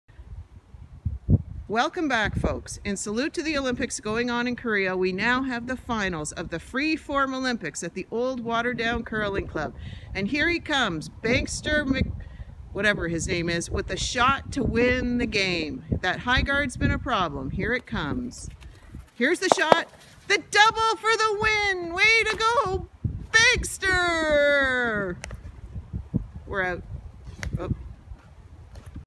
Waterdown #2
Singles Curling at the Old Waterdown Curling Club